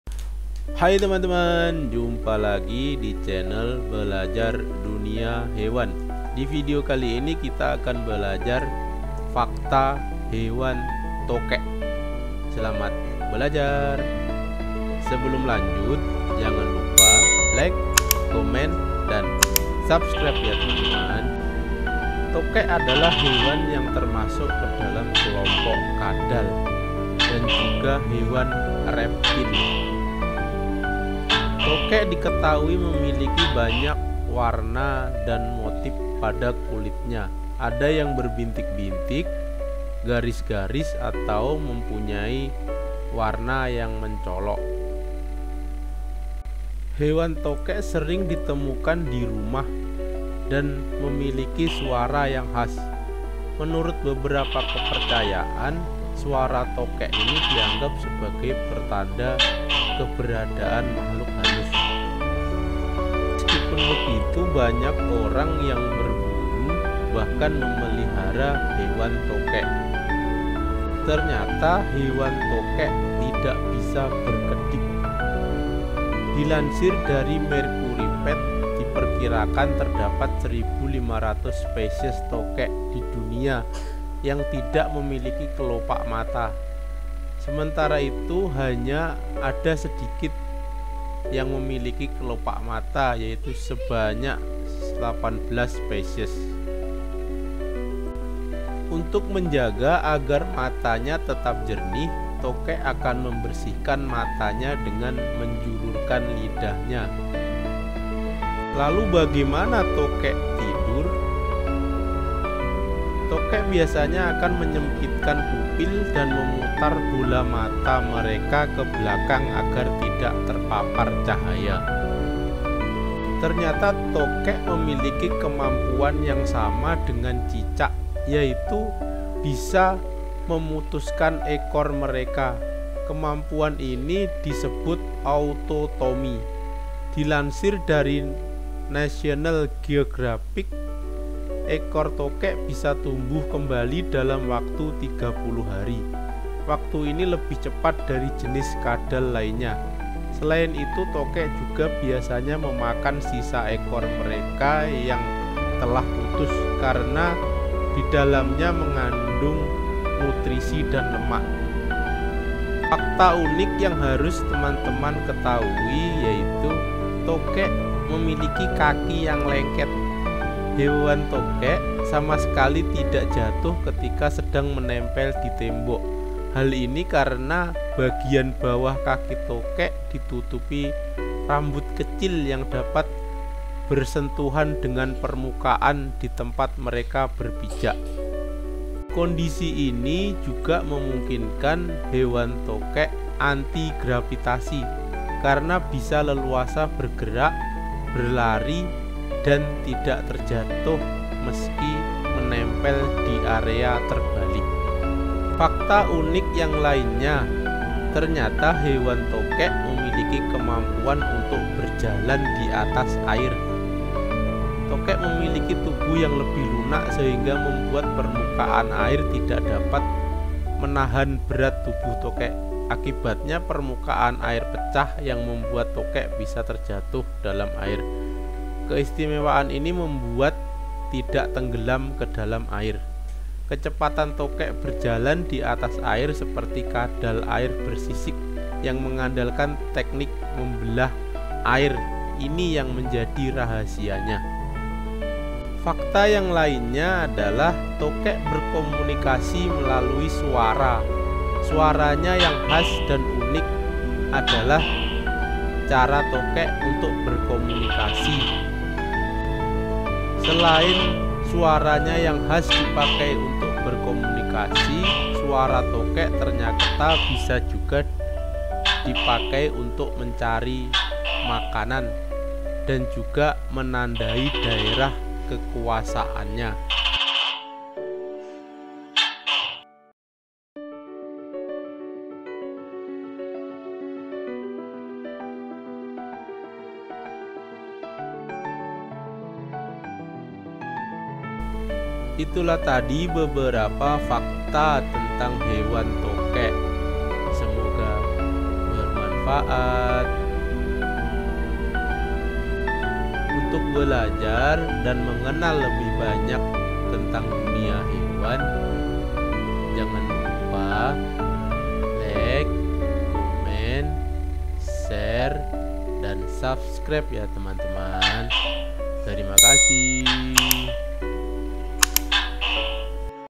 Fakta Tokek
Tokek adalah nama umum untuk menyebut cecak besar atau jenis-jenis kadal dari suku Gekkonidae. Namun istilah tokek secara sempit biasa merujuk pada jenis-jenis dari genus Gekko, famili Gekkonidae. Sedangkan tokek dalam pemahaman orang awam merujuk kepada tokek rumah (Gekko gecko), yang memiliki agihan yang luas dan sangat sering dijumpai di sekitar lingkungan manusia.